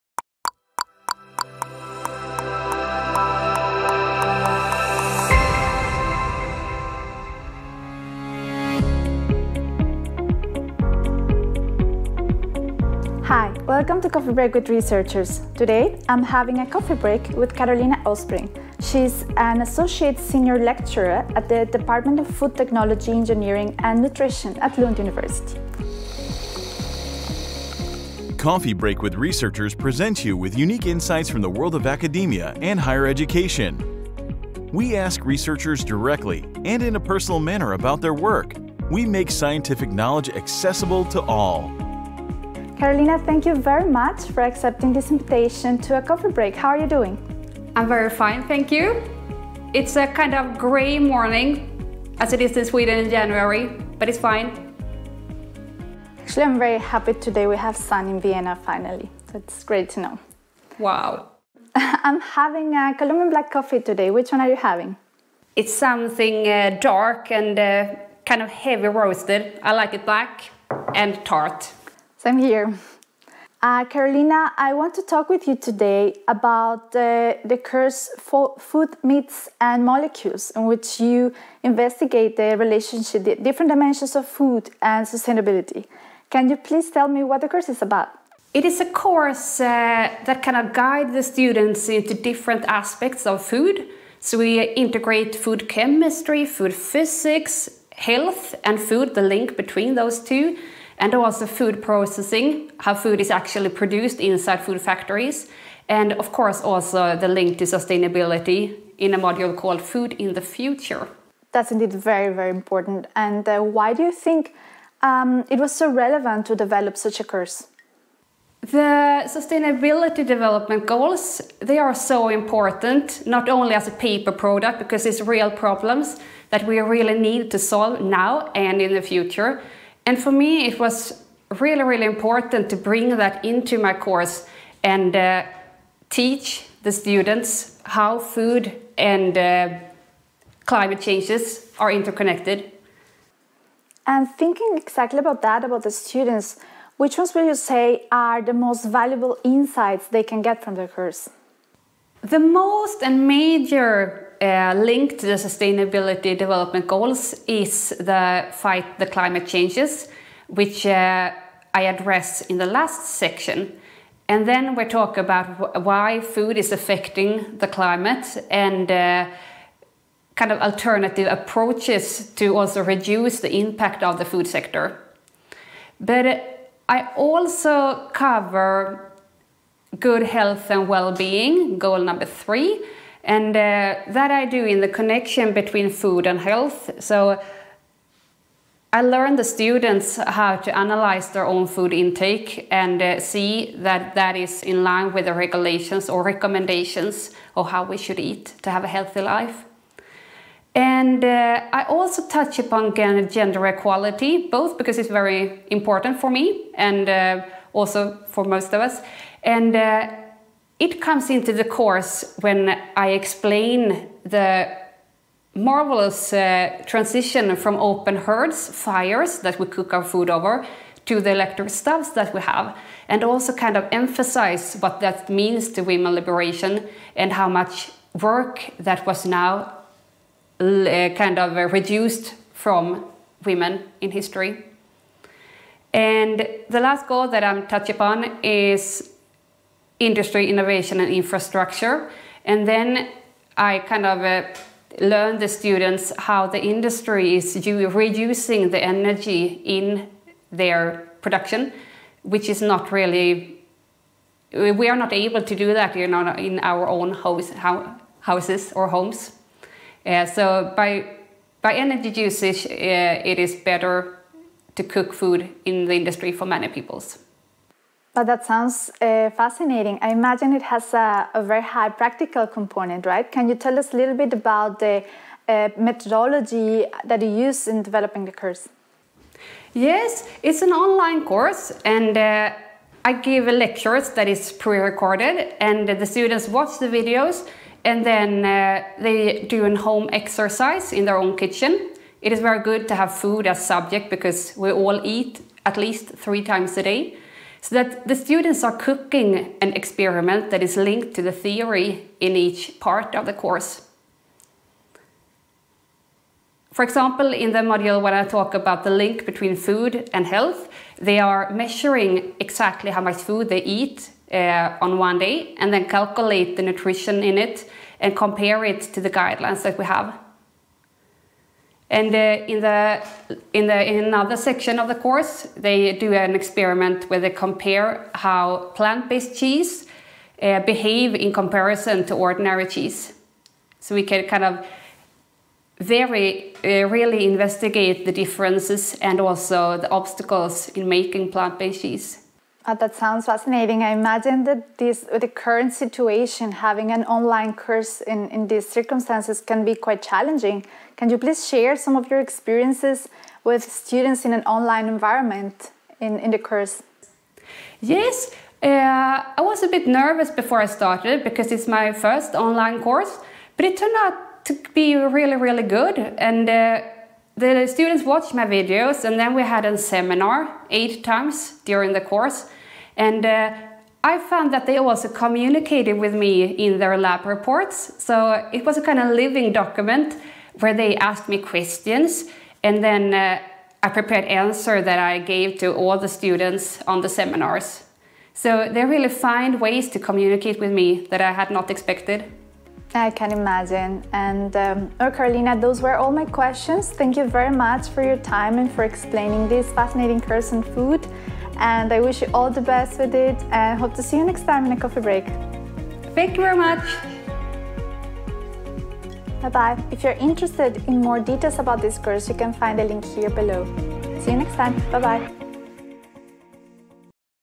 Karolina Östbring - Food, myths and molecules- SustainableDevelopment Goals
Coffee break with Karolina Östbring,  Associate senior lecturer at the Department of Food Technology, Engineering and Nutrition at Lund University

In this interview, Karolina Östbring talks about her course "Food, myths and molecules". She describes how her course connects food technology with several of the Sustainable Development Goals (SDGs) and how students experiment in their own kitchen.